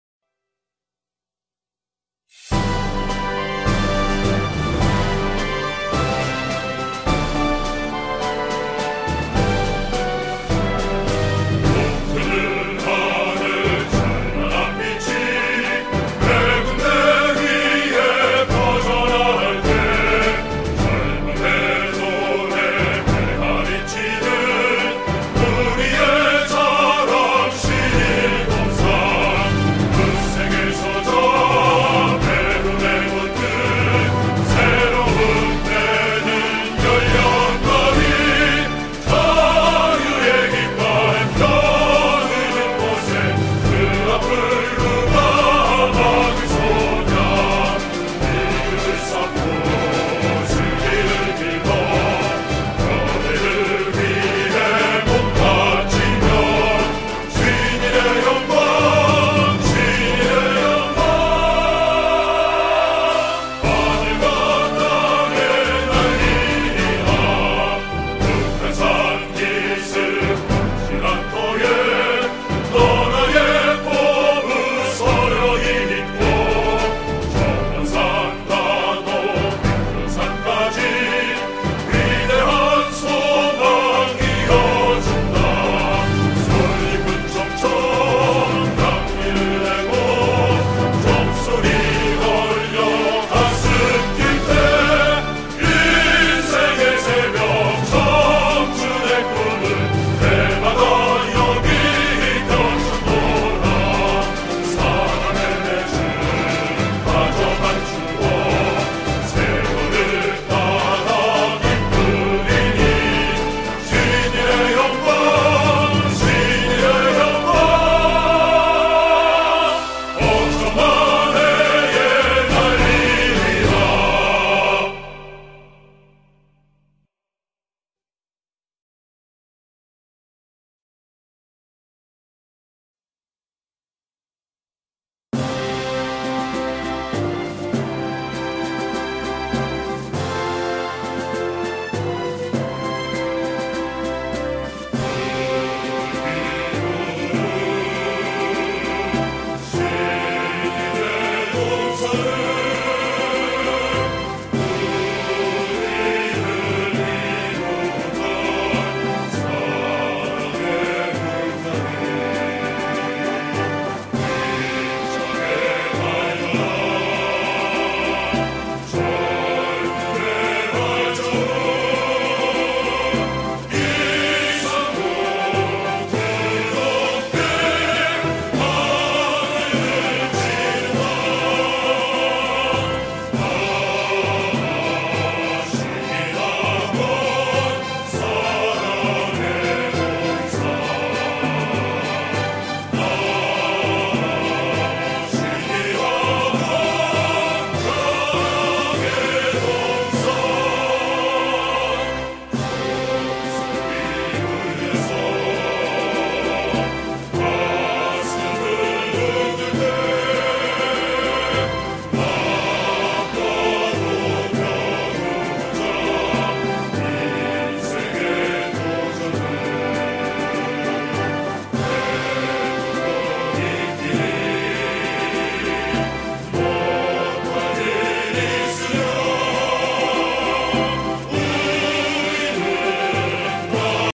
Sinsarang 40th Anniversary-Teachers Treat Day
19 Teachers kindly come over to the party on 5th August 2013 to celebrate 40th anniversary  of the Class 1973, Shin-il High School.